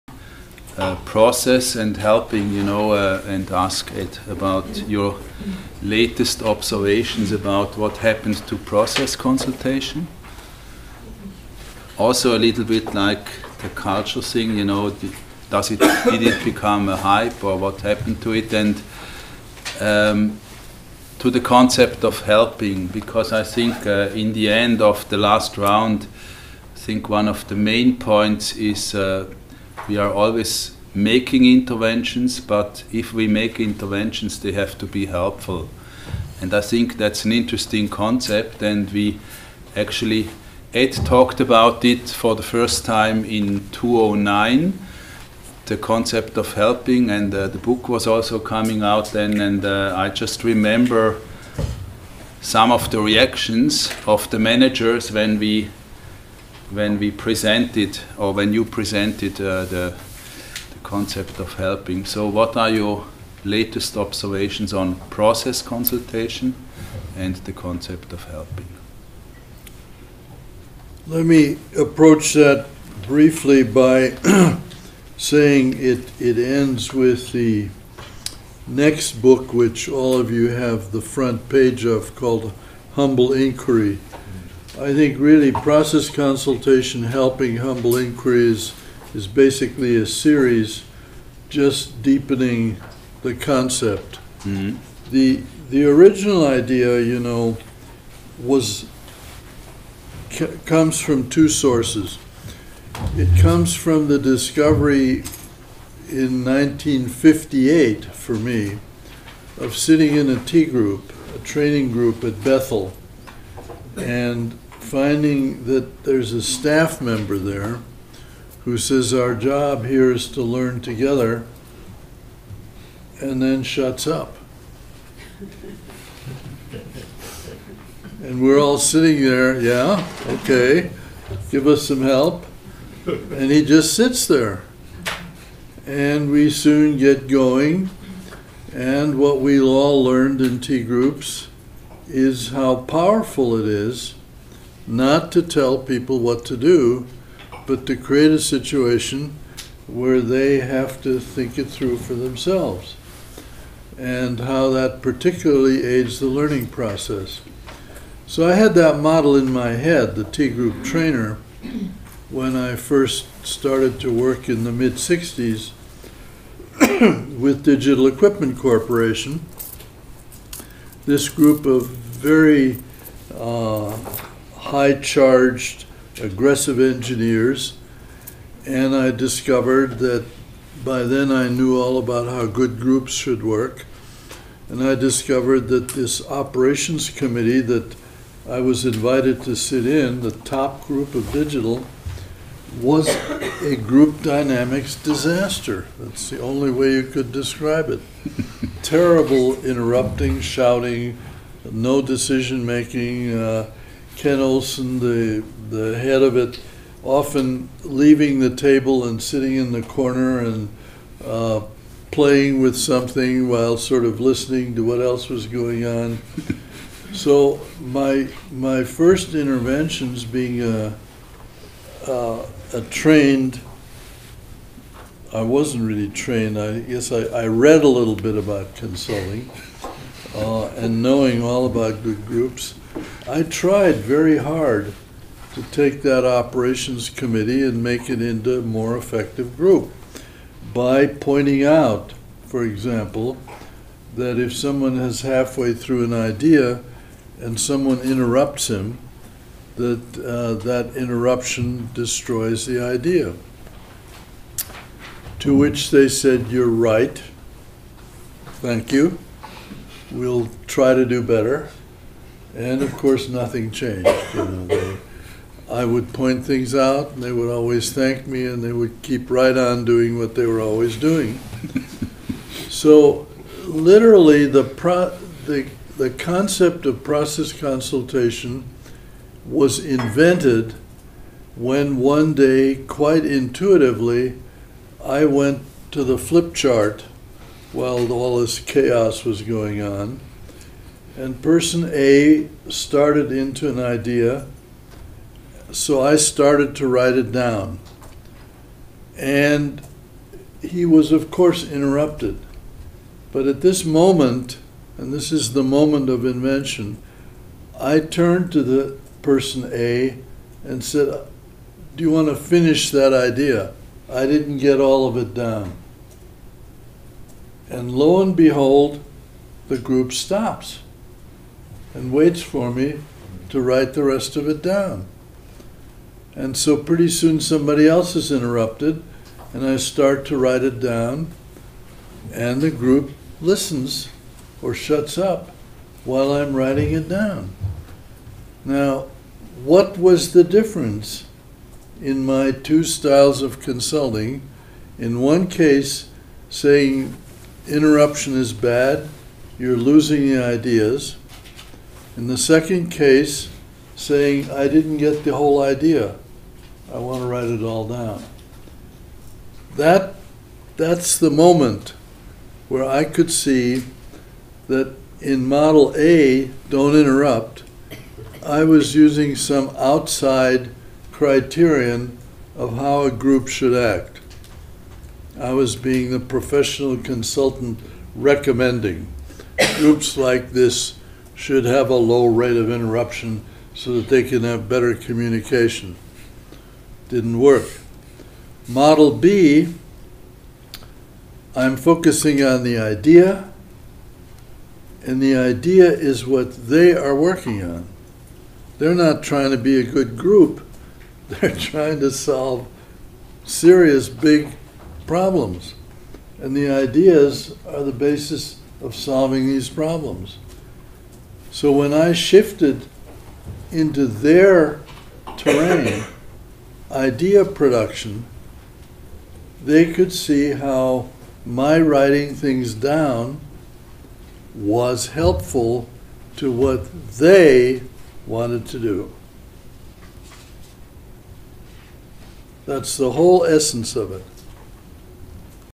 Edgar H. Schein on how the Concepts of 'Process Consultation' and 'Helping' were invented
Video excerpt from the TRIAS Workshop on 'Culture, Process and Helping' on June 24, 2013 at the IBM Zurich Research Center in Rüschlikon. Edgar H. Schein, Professor Emeritus, MIT Sloan School of Management, outlines on how the Concepts of 'Process Consultation' and 'Helping' were invented giving a sneak preview on his new book: 'Humble Inquiry'.